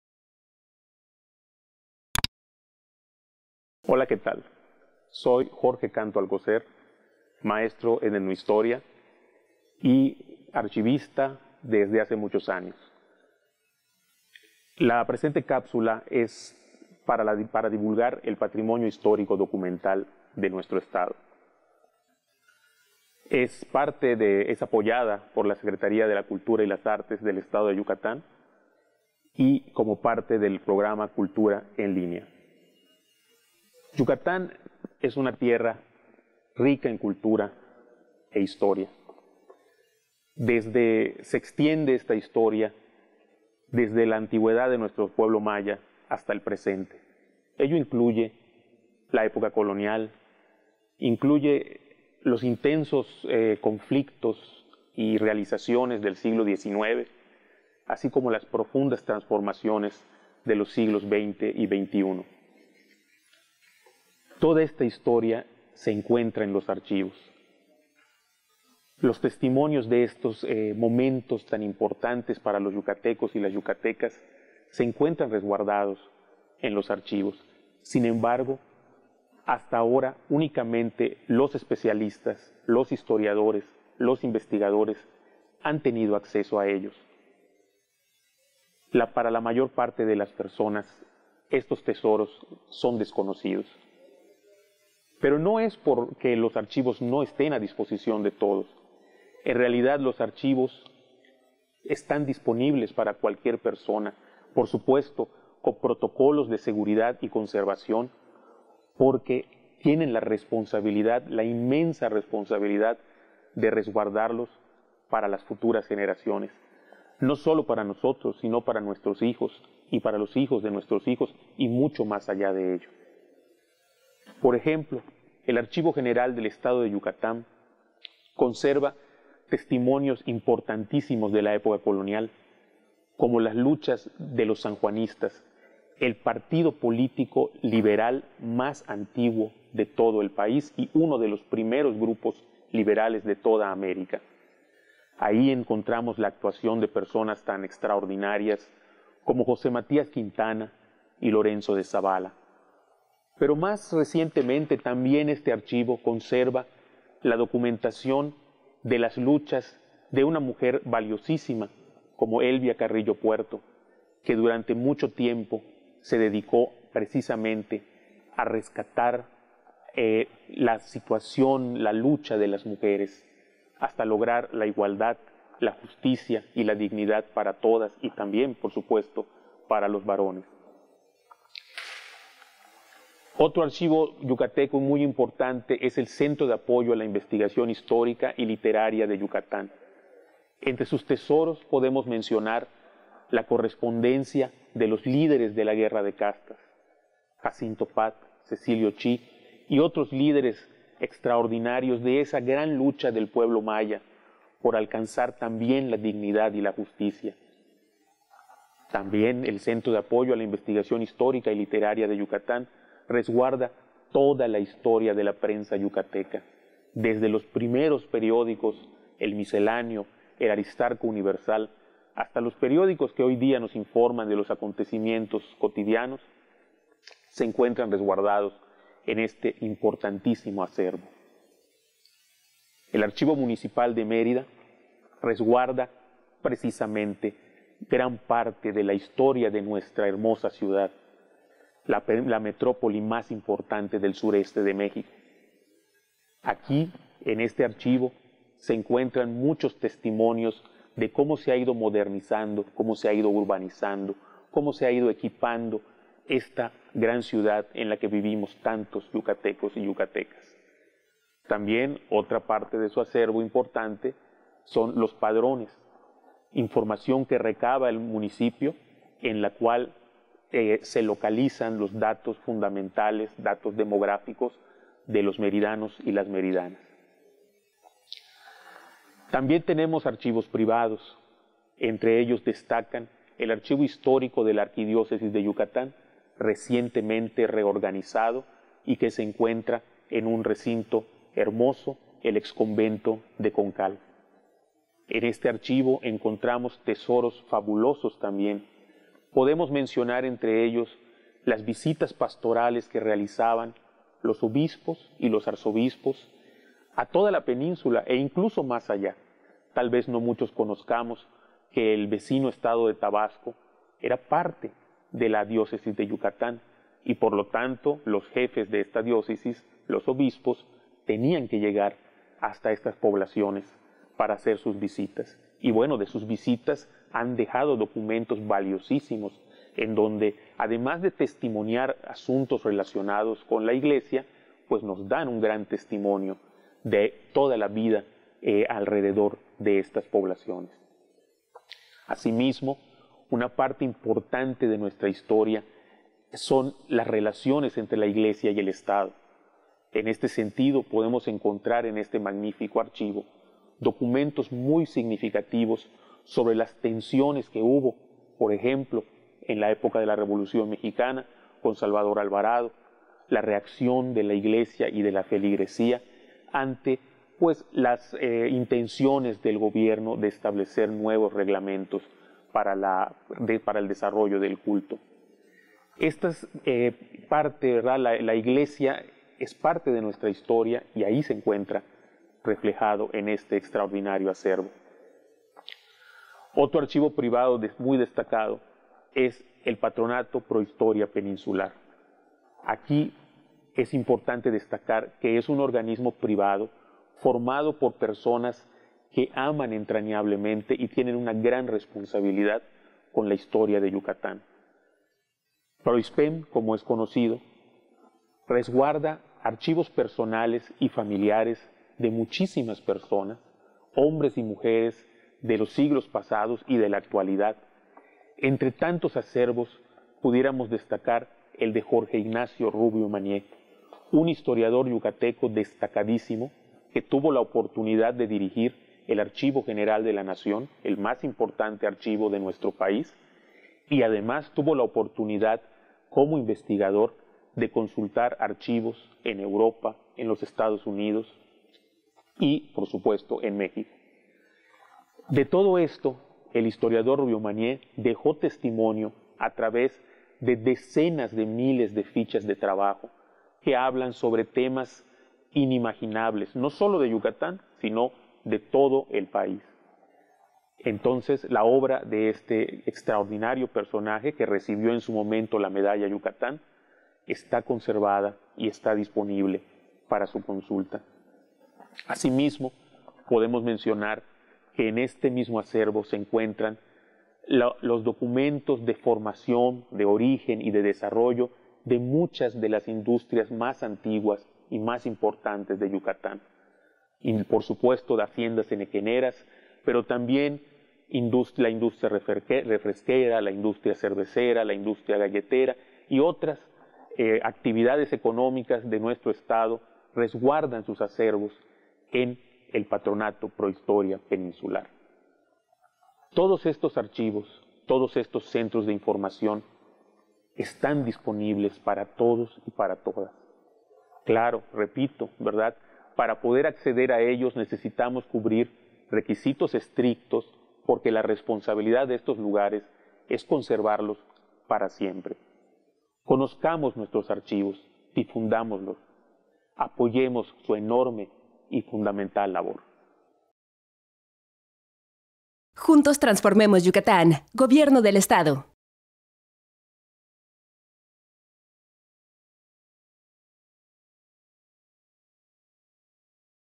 Cápsula: Divulgación del patrimonio histórico y documental de Yucatán [Cultura En Línea-AIEC2020]
En esta Cápsula de CulturaEnLínea, el historiador e investigador Jorge Canto Alcocer, reseña las instituciones que resguardan el acervo histórico y patrimonial en el estado, y las señala a detalle.